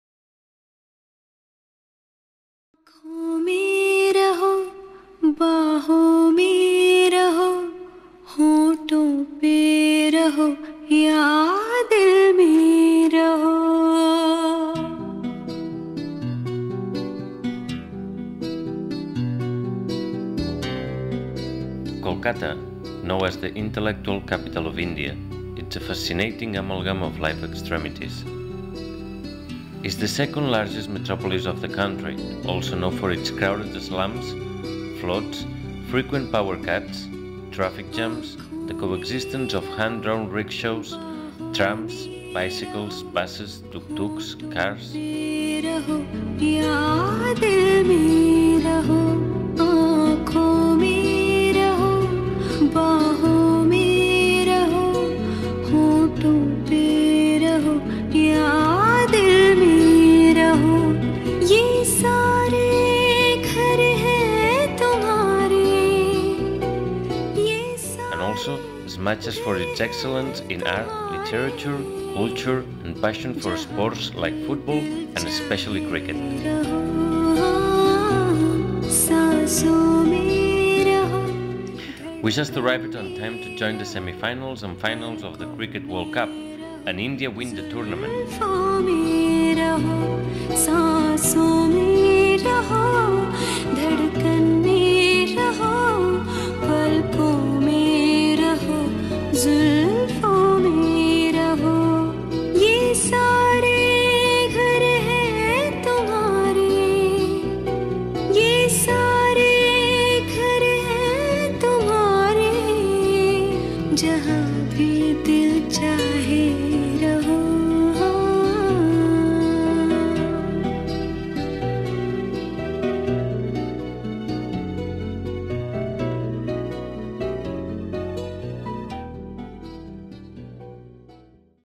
Kolkata, India.mp4
Our first week in Kolkata, mainly spent at the airport trying to release our bicycle batteries